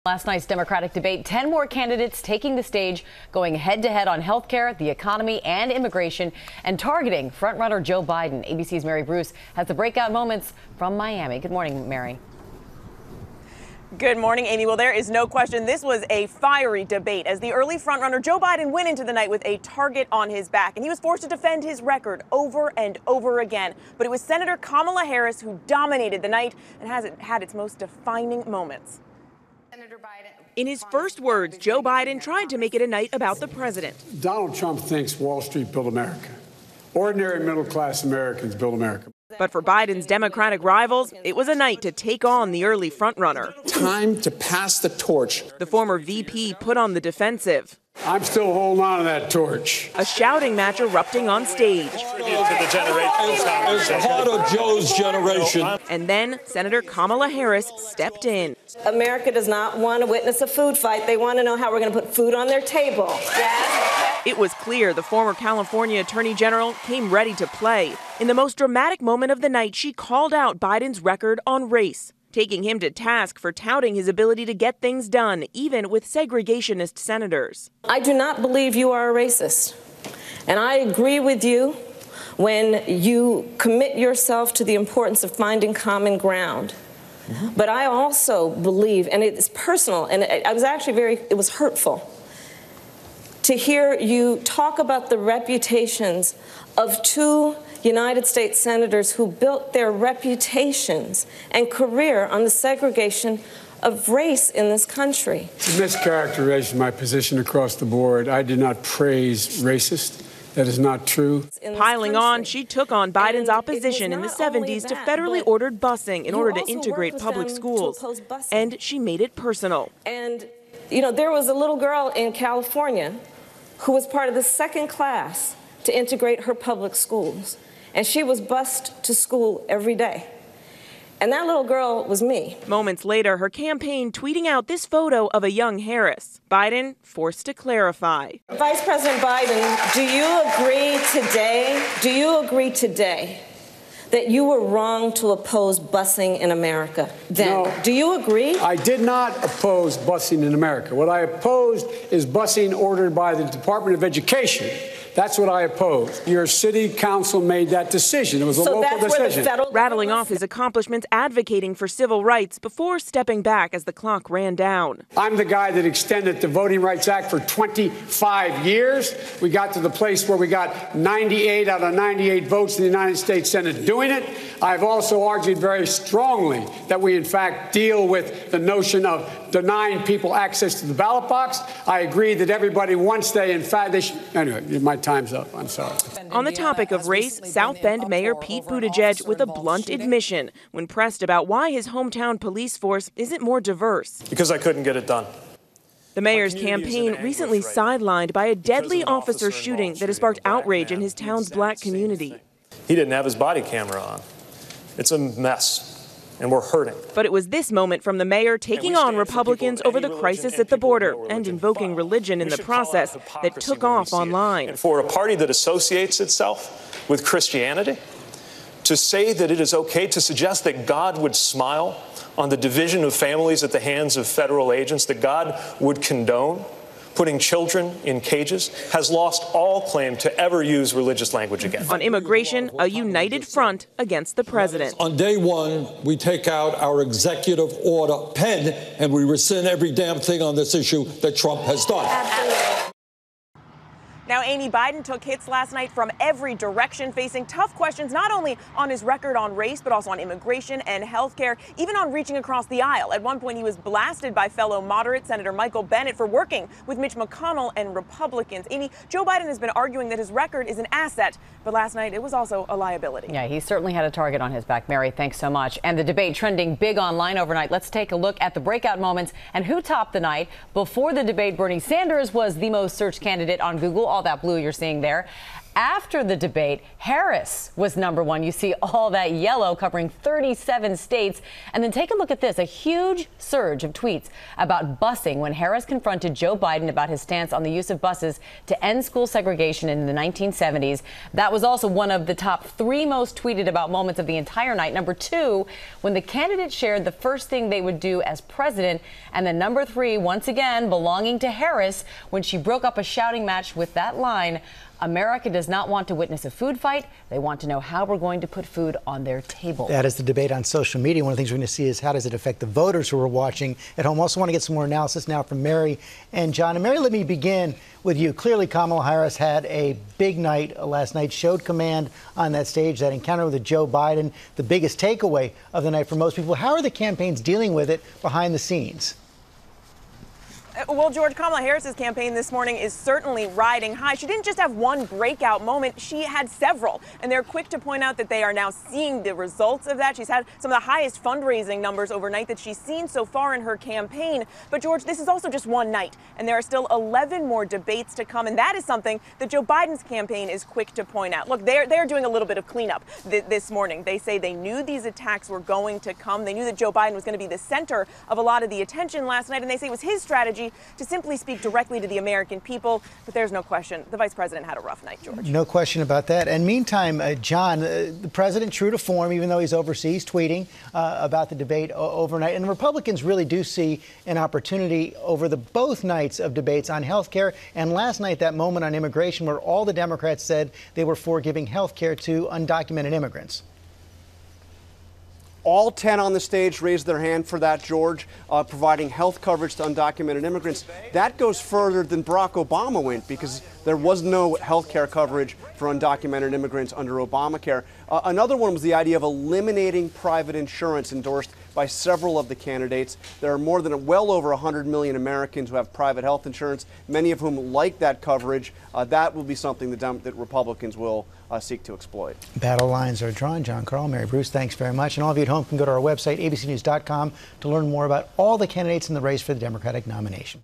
Biden and Harris face off on second night of debates | ABC News
The second group of 10 Democrat candidates went after each other on everything from age to race, including a feisty challenge between Sen. Kamala Harris and former Vice President Joe Biden.

2nd Democratic debate highlights: What you need to know

Harris says she was 'surprised' by Biden's answer on busing after tense exchange at Democratic debate